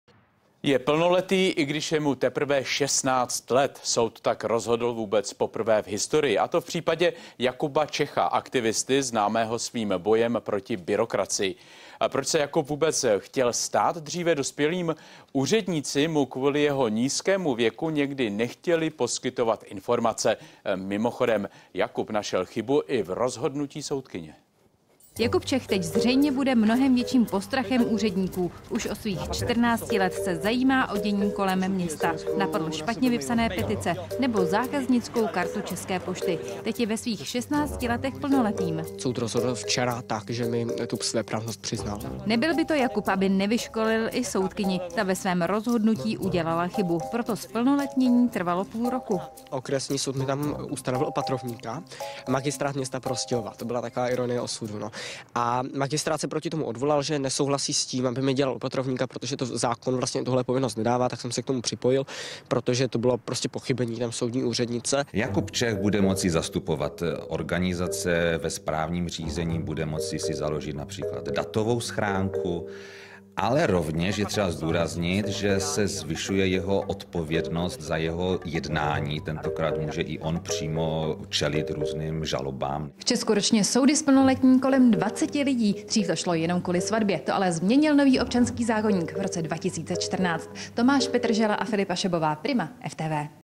Jakub Čech získal v 16 letech svéprávnost - reportáž TV Prima
UPŘESNĚNÍ: Jakub Čech nevyškolil soudkyni, ta ani neudělala chybu. Tu udělala před časem soudní úřednice a upozornil ji na to i prostějovský magistrát, který se proti tomuto pochybení odvolal.
Příběh aktivisty Jakuba Čecha, který i přes svůj nízký věk několikrát vyškolil úředníky, se dočkal zásadního zlomu. Věk by pro nadšeného šestnáctiletého bojovníka, který v boji proti byrokratické zvůli nepolevil, už nikdy neměl být překážkou. Podle rozhodnutí Okresního soudu v Prostějově se stal pně svéprávným.